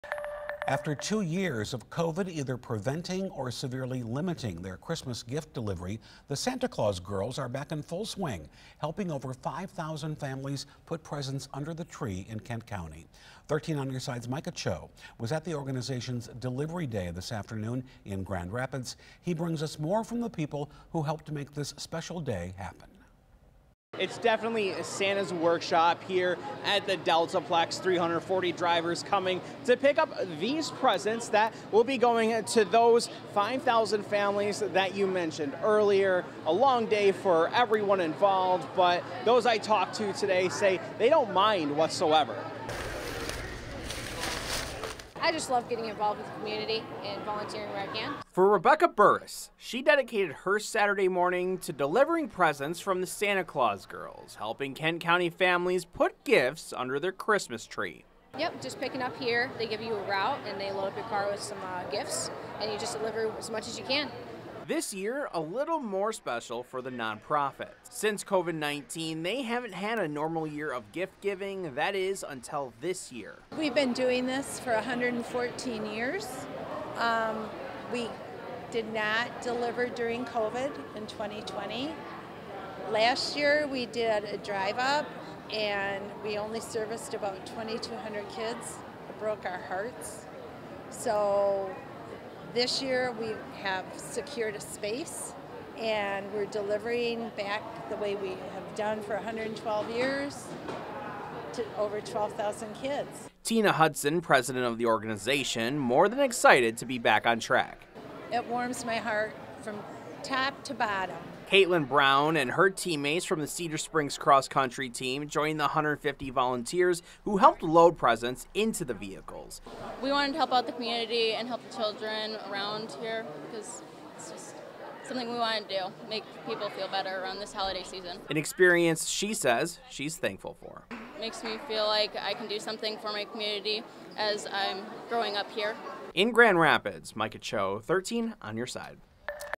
'It warms my heart from top to bottom': Hundreds show up to help Santa Claus Girls' mission
For Rebekah Burris, she dedicated her Saturday morning to driving presents for The Santa Claus Girls, helping Kent County families put gifts under their Christmas Tree.

"I just love getting involved with the community and volunteering where I can," said Burris.